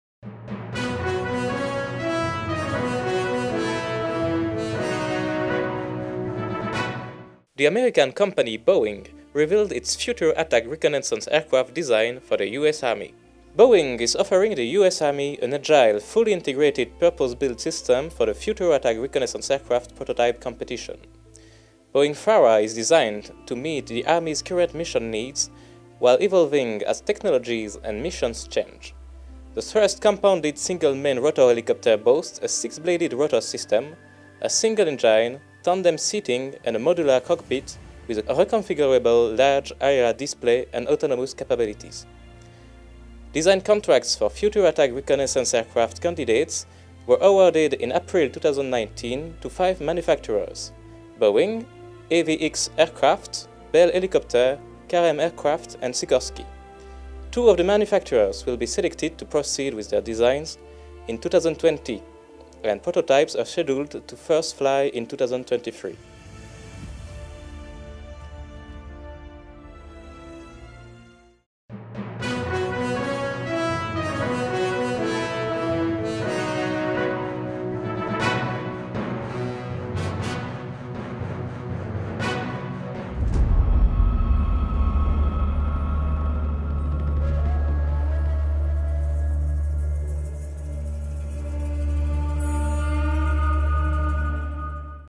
Boeing reveals its FARA Future Attack Reconnaissance Aircraft design for US Army
Boeing is offering the U.S. Army an agile, fully integrated, purpose-built system for the Future Attack Reconnaissance Aircraft (FARA) prototype competition.